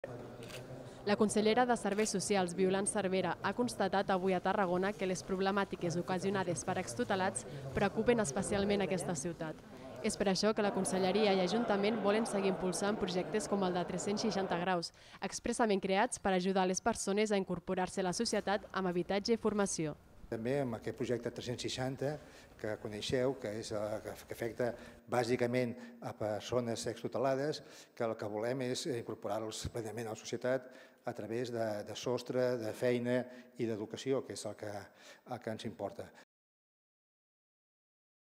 La Conselleria de Serveis Socials i Tarragona s'uneixen per afrontar la problemàtica dels extutelats
Volen seguir impulsant projectes com el de 360º, expressament creats per ajudar a les persones a incorporar-se a la societat amb habitatge i formació.